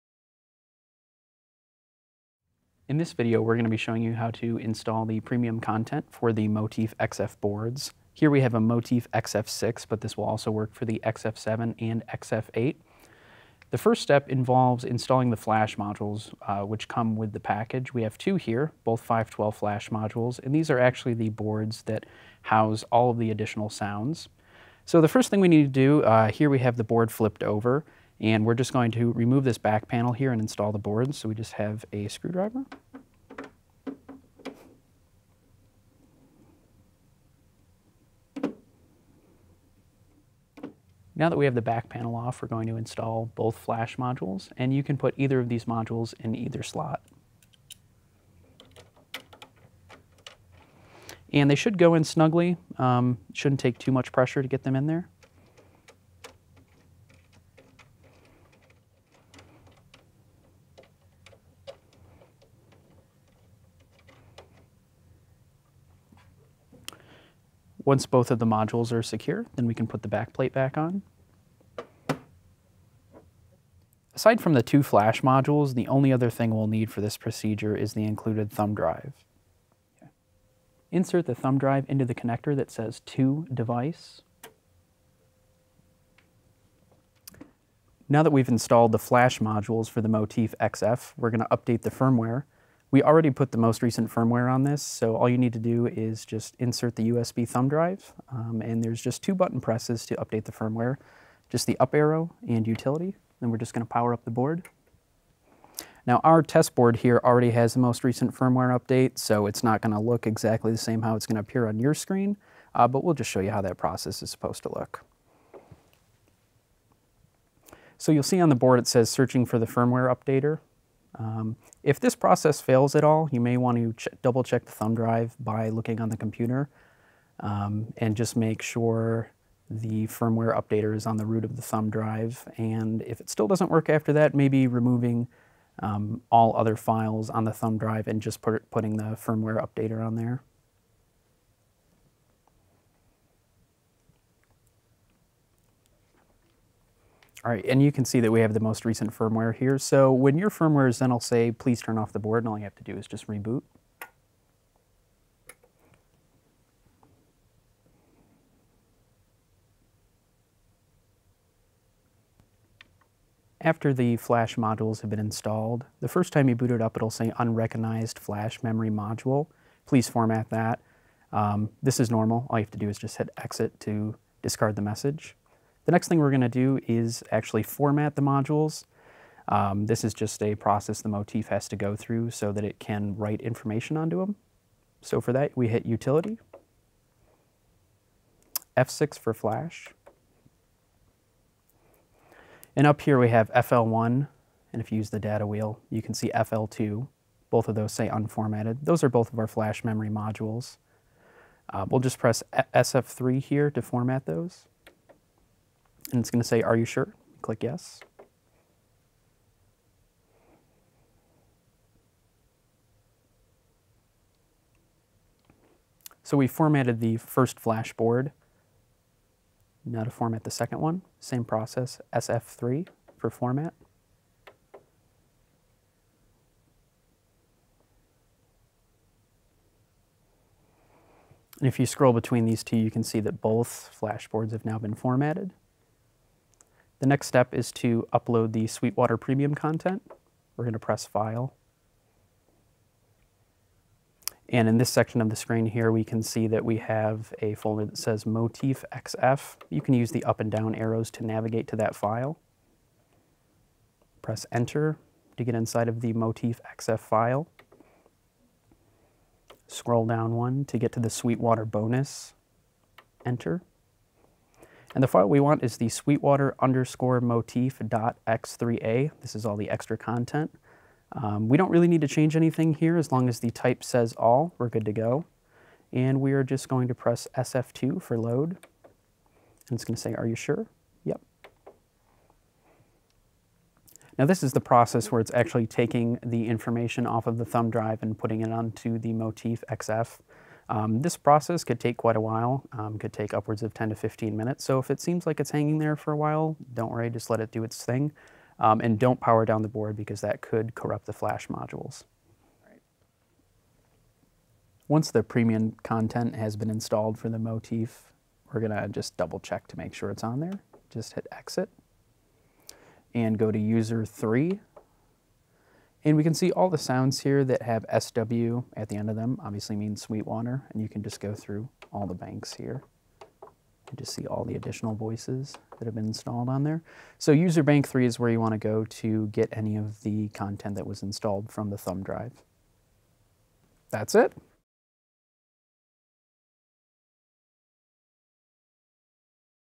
Tech Tip: Installing the Yamaha MOTIF Premium Content
This edition of Sweetwater Tech Guides will show you the process for installing the Yamaha MOTIF Premium Content including the physical installation of the flash boards as well as how to update the MOTIF's firmware and write the premium content from the USB drive to the flash boards.